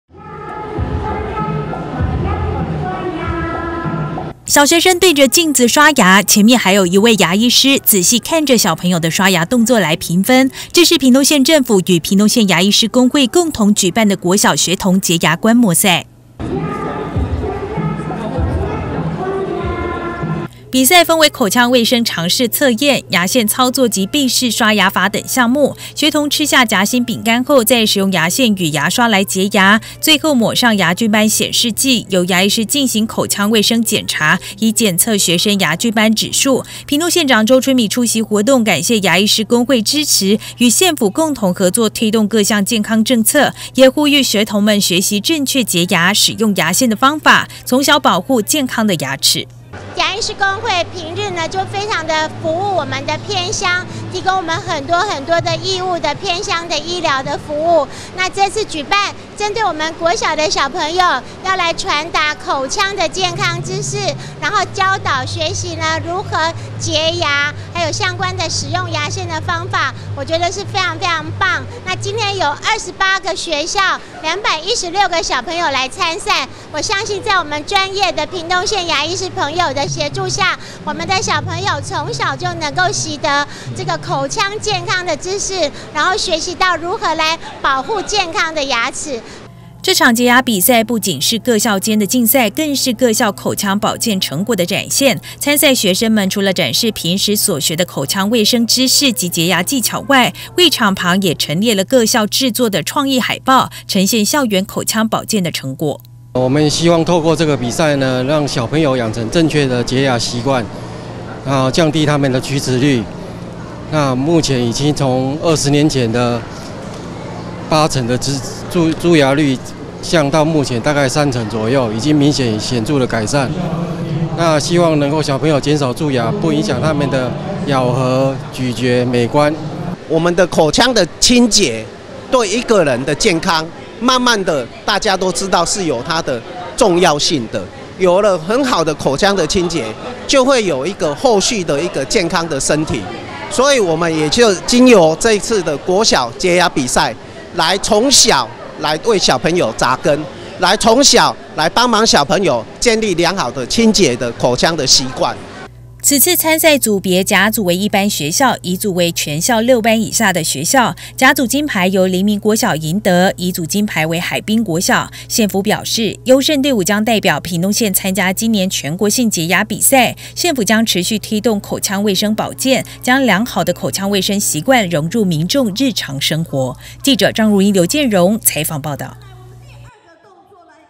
屏縣辦理國小學童潔牙觀摩賽 推動口腔保健工作
屏縣學生齲齒率逐年下降，雖高於全國平均值，但差距已持續遞減，屏東縣政府持續推動學童口腔保健工作，與屏東縣牙醫師公會在仁愛國小舉辦國小學童潔牙觀摩賽，共28校、216位學生報名參加，展現推動口腔保健的成果。